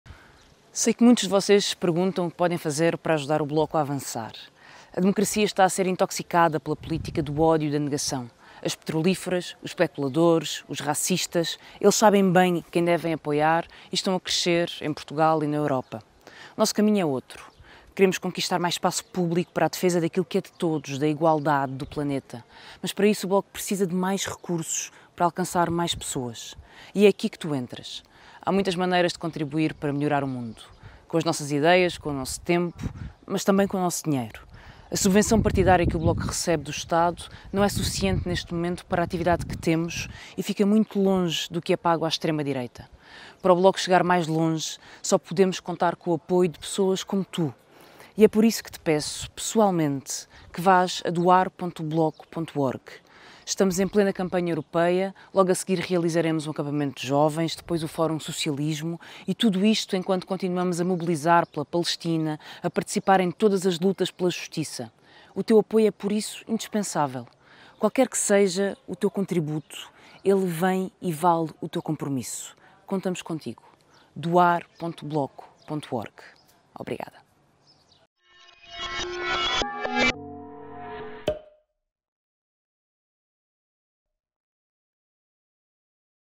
Só podemos contar com o teu apoio
A democracia está a ser intoxicada pela política do ódio e da negação. Petrolíferas, especuladores e racistas sabem quem devem apoiar.

Pelo nosso lado, queremos conquistar espaço público na defesa do que é de todos, da igualdade e de direitos.

Precisamos de mais meios para chegar a mais gente e só podemos contar com o apoio de pessoas como tu.